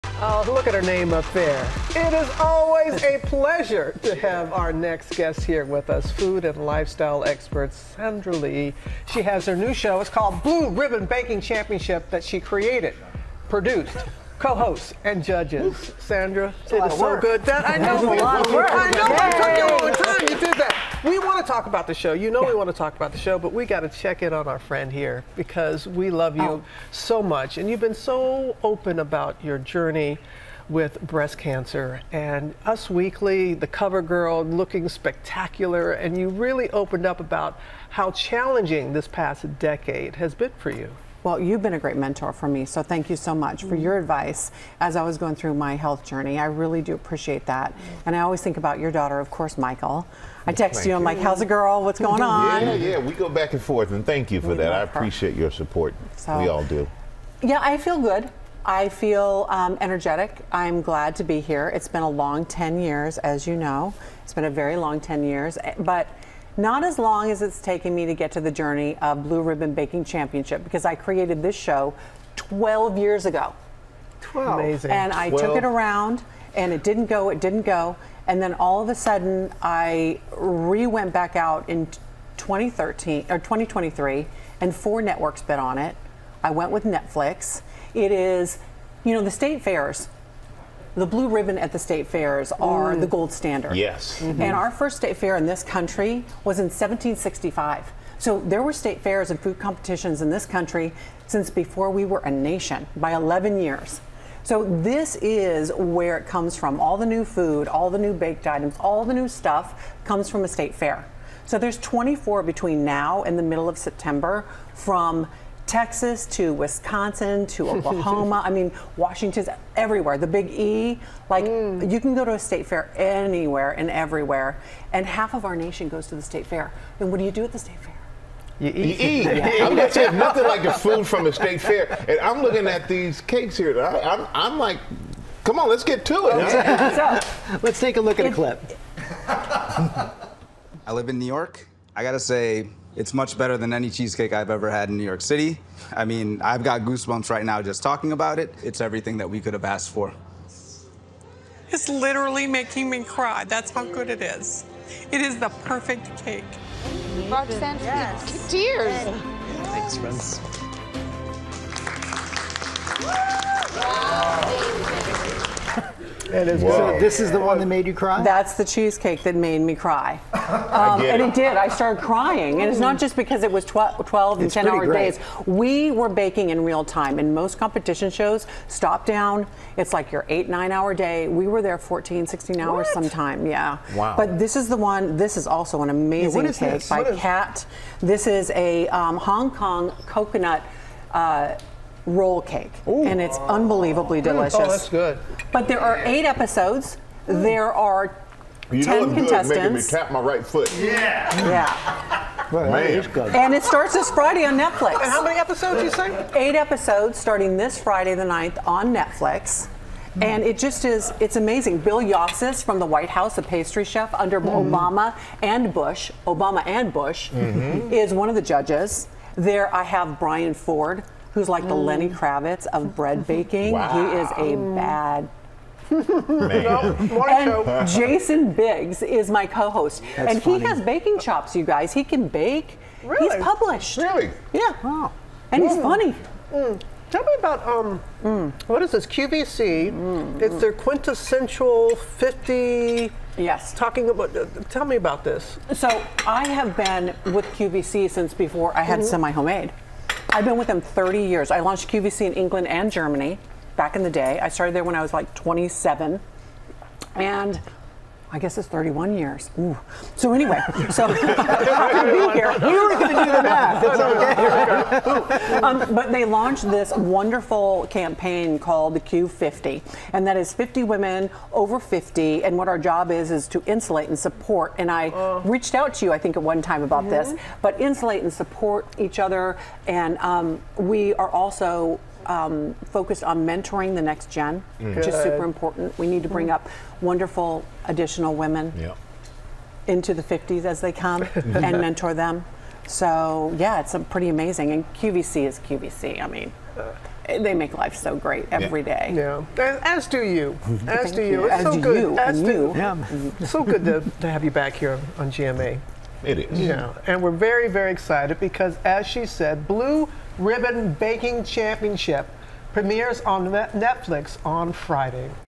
Sandra Lee talks 'Blue Ribbon Baking Championship'
The chef and author discusses the Netflix show about blue ribbon-winning bakers from state fairs across the U.S. who face off to win $100,000.